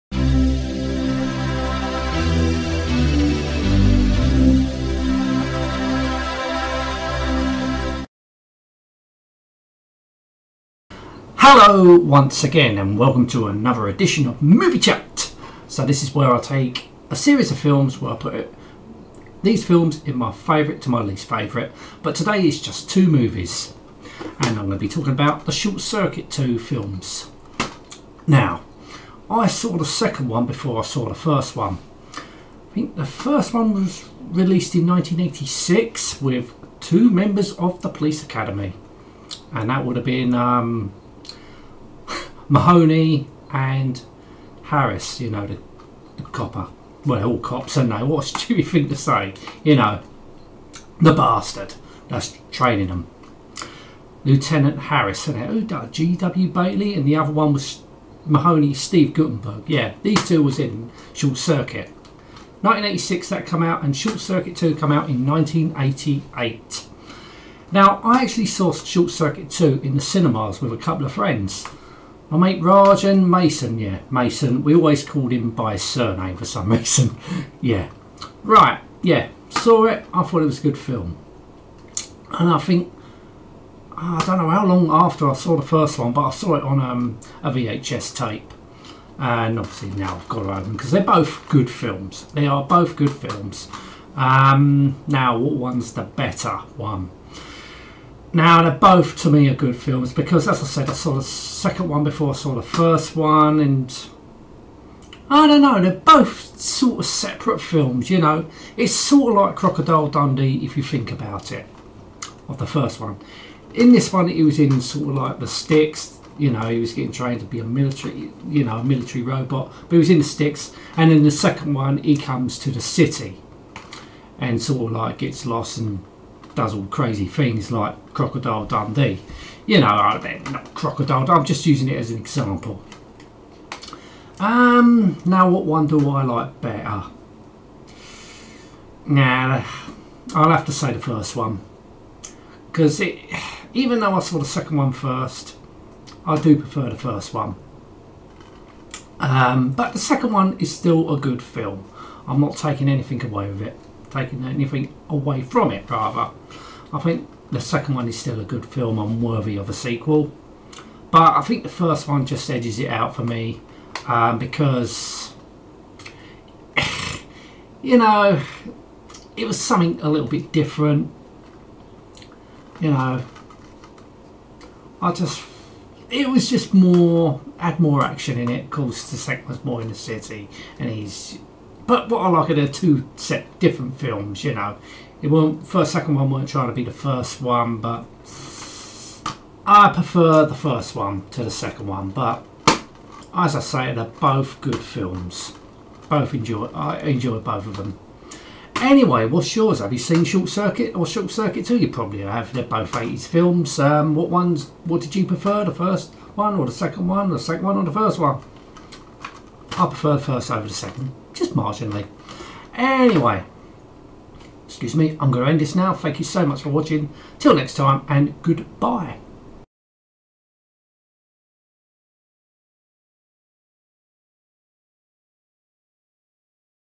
Talking On The Camera 08/05/2021 - Movie Chat (10) - Short Circuit Movie Series
Talking about the order I put the Short Circuit movies into, from my favorite to my least favorite. What are yours? Warning! This video has swearing.